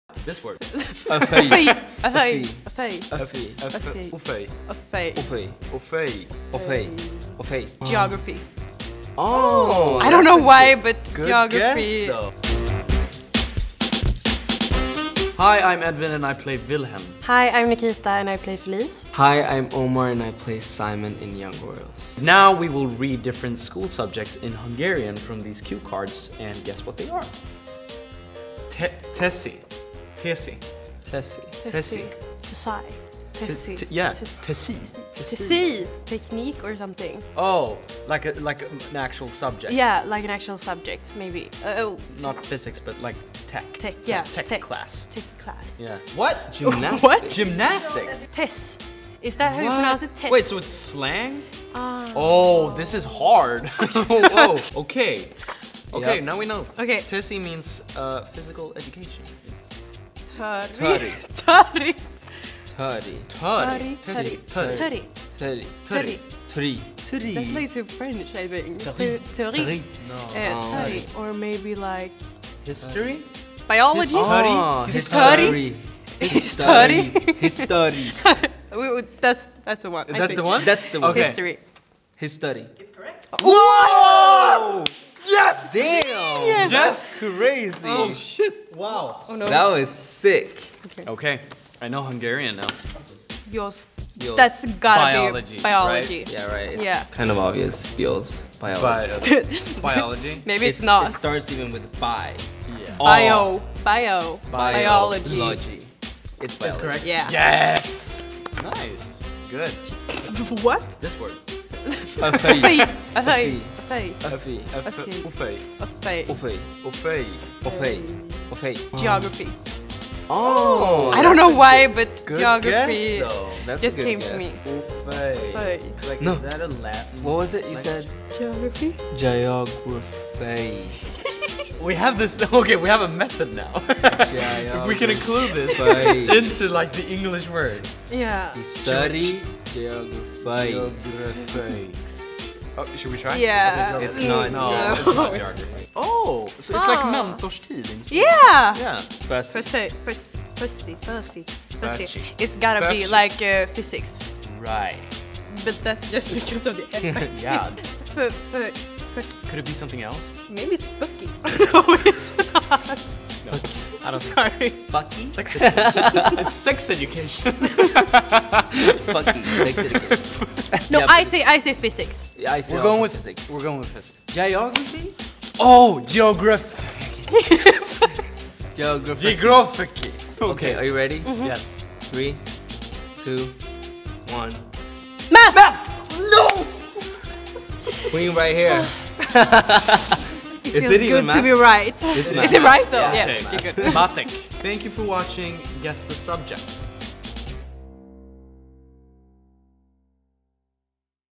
Entrevista Young Royals | Qual a matéria escolar? Com Omar Rudberg e elenco [PT-BR] [Subs en Esp]
© All the rights to Netflix Hungary

Edvin Ryding, Nikita Uggla e Omar Rudberg brincam de "Qual a matéria escolar?"

Legendas PT-BR no [CC]

Active [CC] para los subtítulos en español.